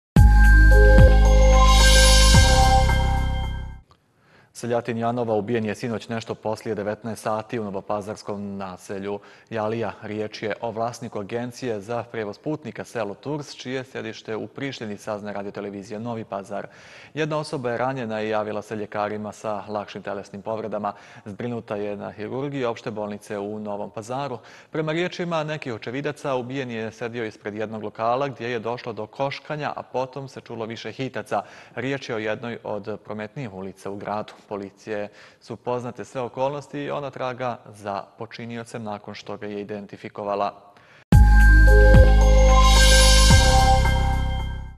Ubistvo u naselju Jalija
Seljatin Janova ubijen je sinoć nešto posle 19 sati u novopazarskom naselju. Reč je o vlasniku agencije za prevoz putnika “Selo tours” čije je sedište u Prištini,  saznaje RTV Novi Pazar. Jedna osoba  je ranjena i javila se lekarima sa lakšim telesnim povredama. Zbrinuta je na na hirurgiji Opšte bolnice u Novom Pazaru. Prema rečima nekih očevidaca ubijeni je sedeo ispred jednog lokala, gde je došlo do koškanja, a potom se čulo više hitaca. Reč je o jednoj od prometnijih ulica. Policija su poznate sve okolnosti i ona traga za počiniocem.